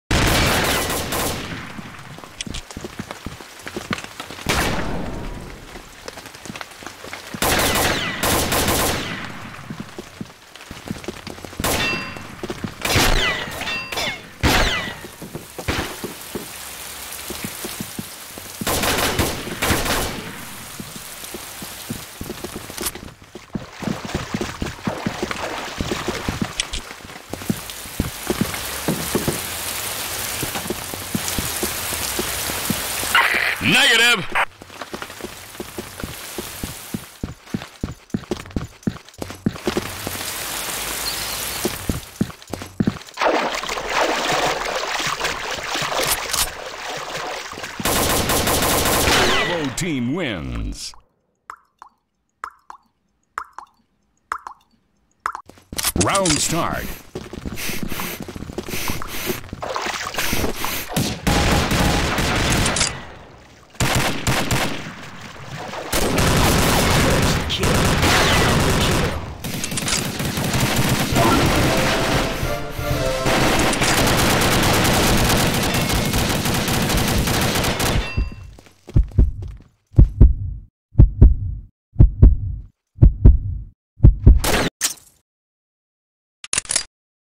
Raged after 3 kills
wtf first game of the day...in 3 days...and i get raged after 1 rush to basement
gg